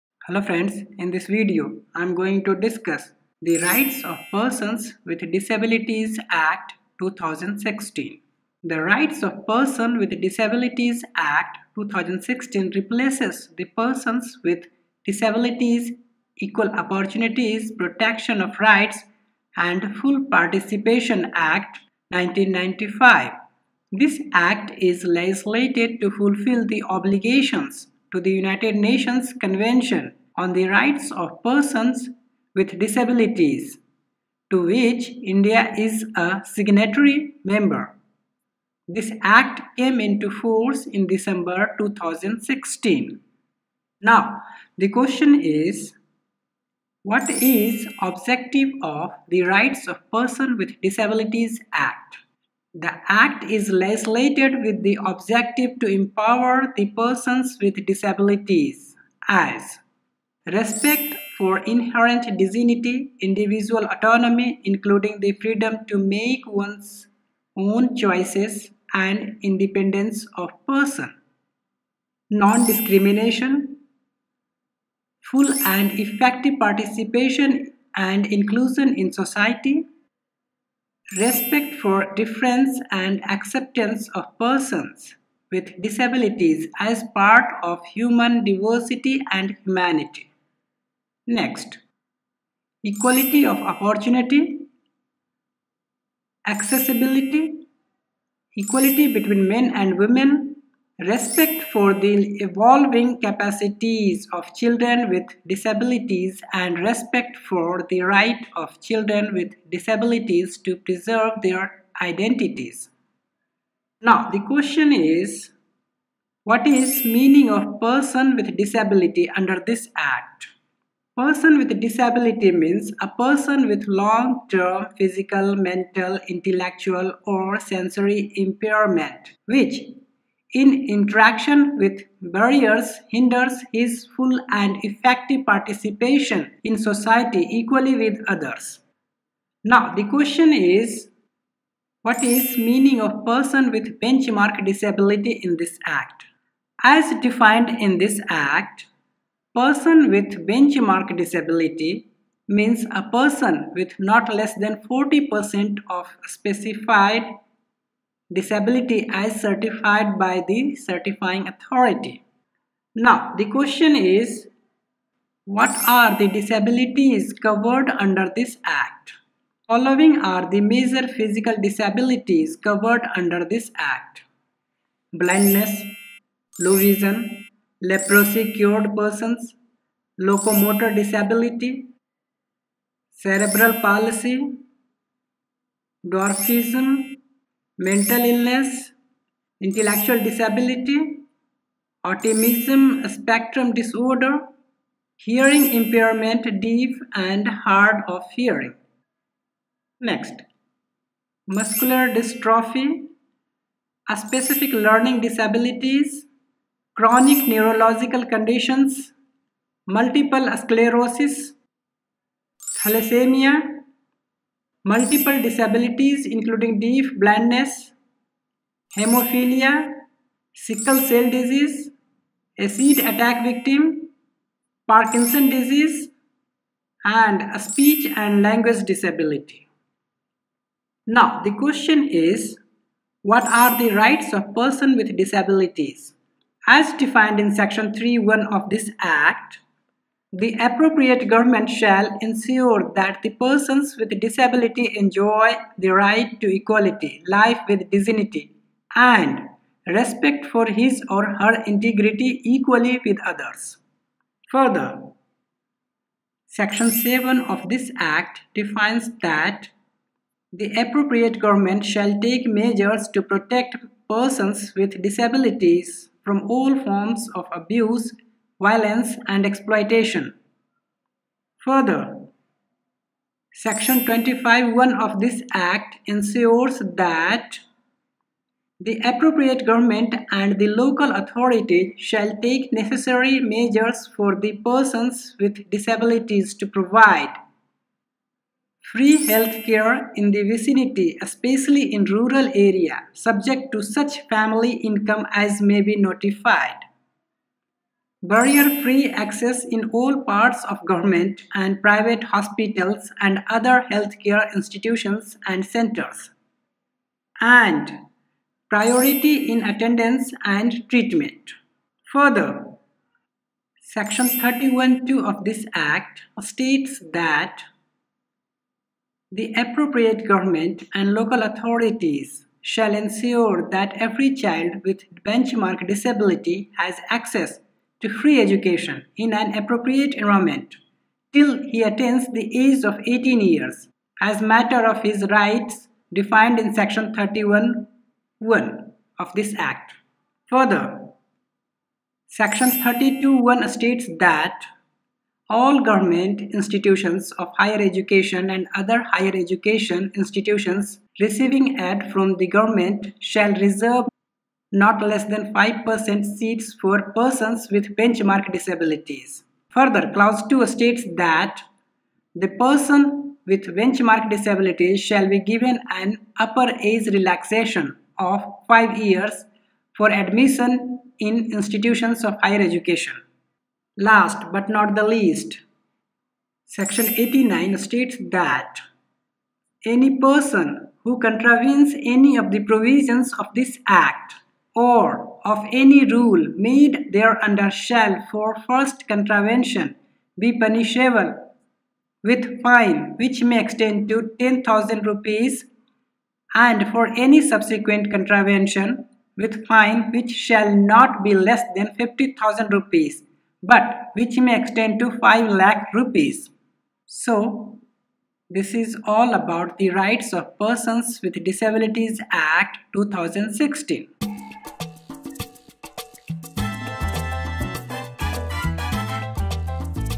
The Rights of Persons with Disabilities Act, 2016
The Rights of Persons with Disabilities Act is legislated to fulfill the obligations to the United Nations Convention on the Rights of Persons with Disabilities (UNCRPD), to which India is a signatory member.  This Act came into force in December 2016.